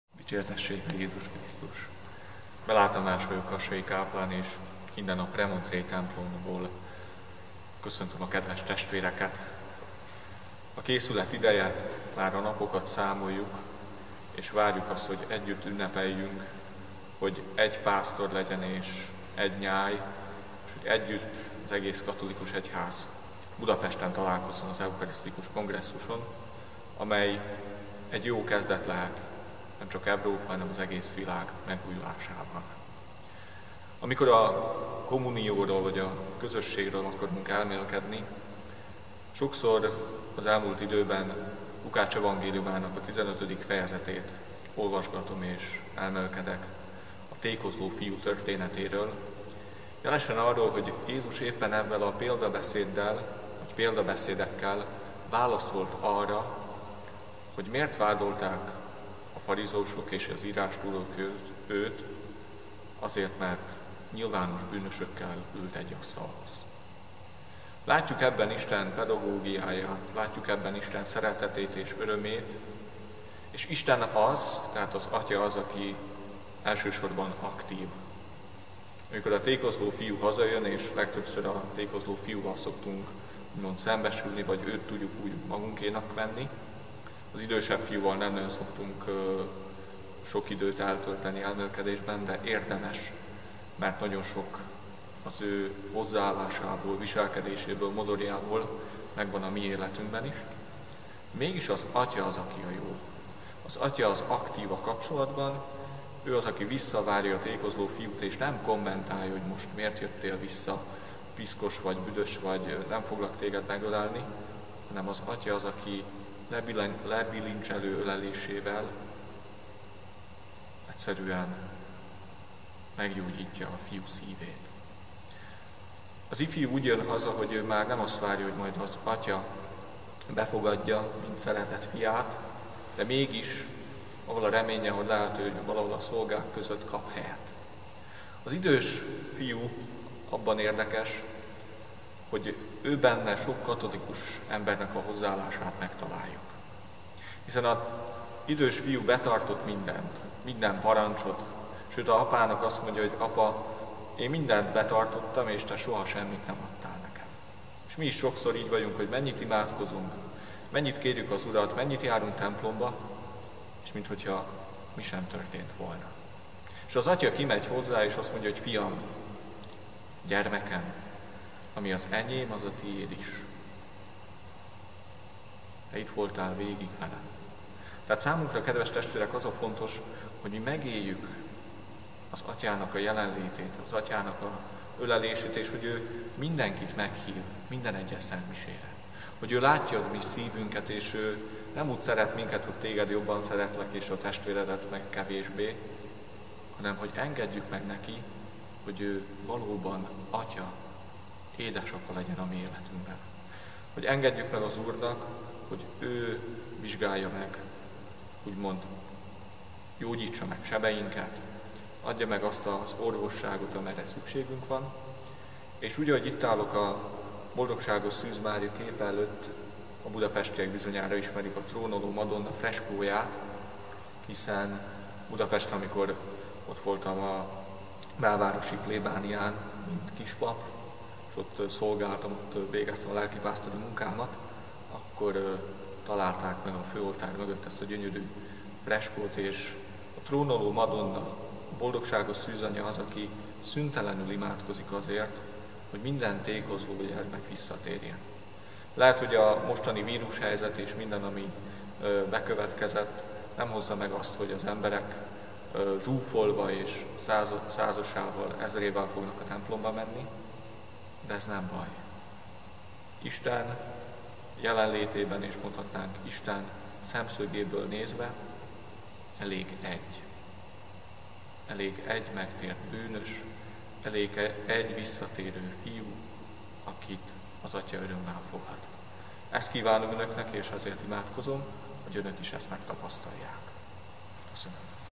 Belá Tamás atya elmélkedése 2021. május 3-án
Minket Krisztus szeretete gyűjtött egybe

Belá Tamás atya gondolatai Kassa városából az 52. Nemzetközi Eucharisztikus Kongresszusra történő lelki felkészülés jegyében.

Még 125 nap a budapesti 52. Nemzetközi Eucharisztikus Kongresszusig.

Kövessen bennünket minden nap!